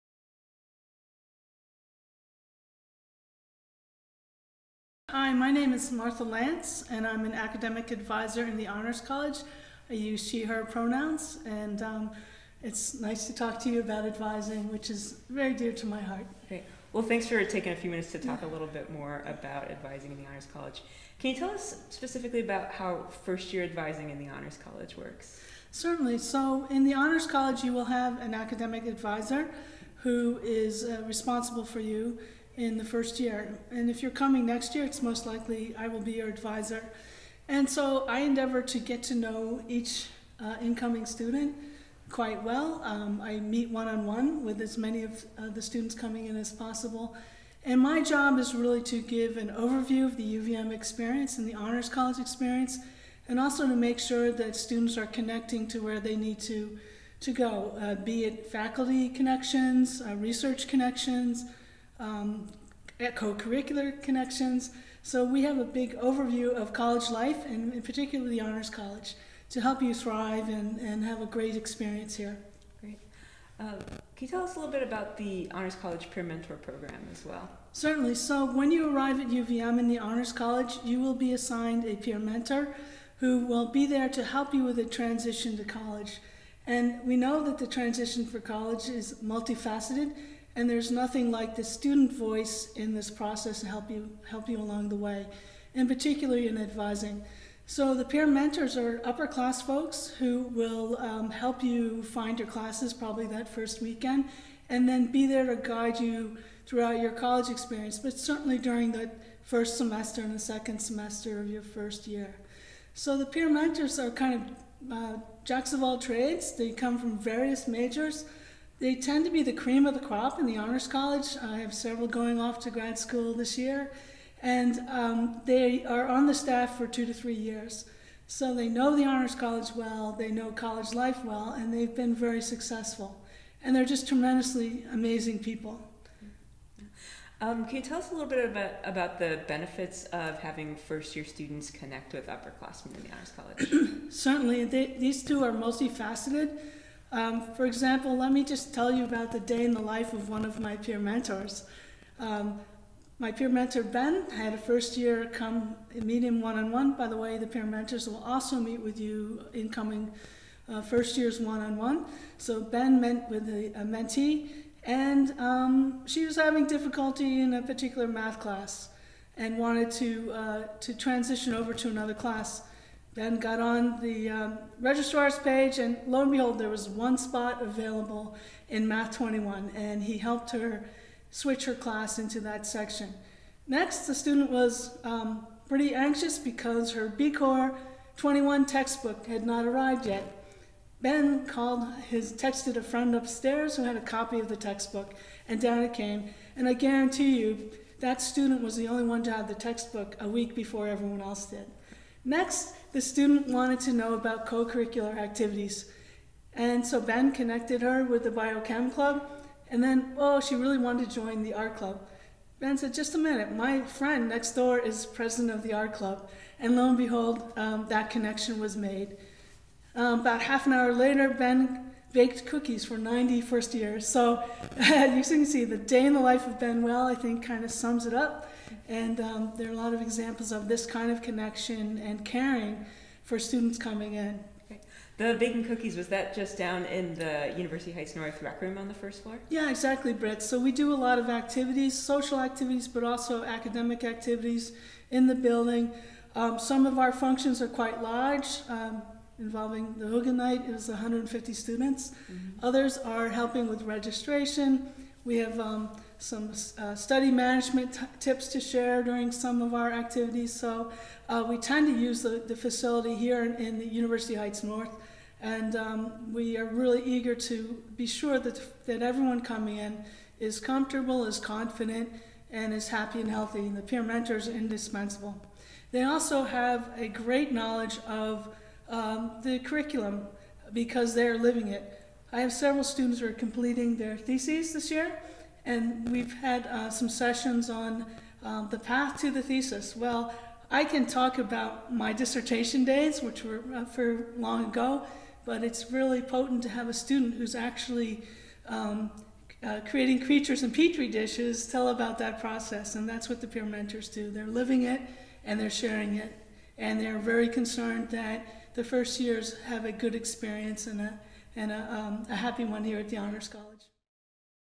Academic Advisor Martha Lance on Honors College Advising and Peer Mentors
Honors College Academic Advisor Martha Lance describes first year Honors advising at the University of Vermont, as well as the Honors College Peer Mentor Program.